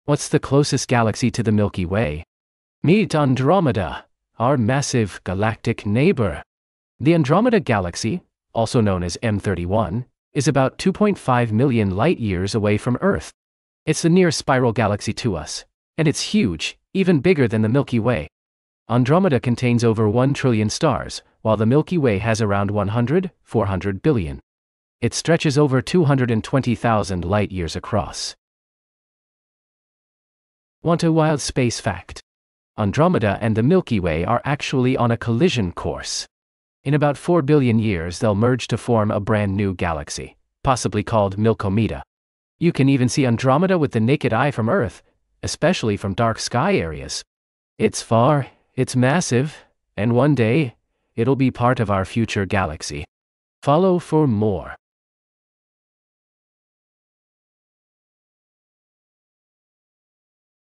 Meet the Andromeda Galaxy: Our Future Galactic Collision!
The Andromeda Galaxy, also known as M31, is the closest spiral galaxy to the Milky Way — and it’s heading straight for us! In this 60-second video, discover mind-blowing facts about Andromeda, including its size, star count, and the epic future collision that will reshape our galactic neighborhood.

⭐ Learn why Andromeda is visible to the naked eye, how big it really is, and what "Milkomeda" might be. Perfect for space fans, students, and curious minds!